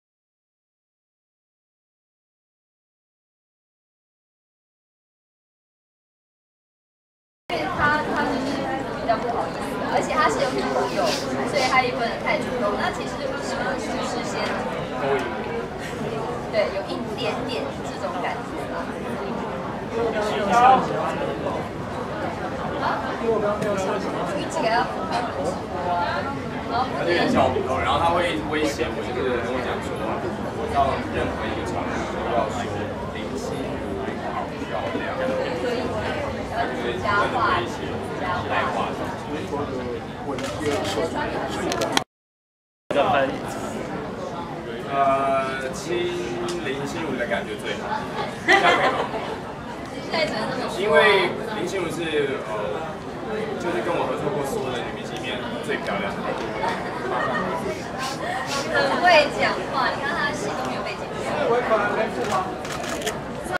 20160512《魔宮魅影》首映 楊祐寧親林心如感覺最好
20160512《魔宮魅影》首映楊祐寧親林心如感覺最好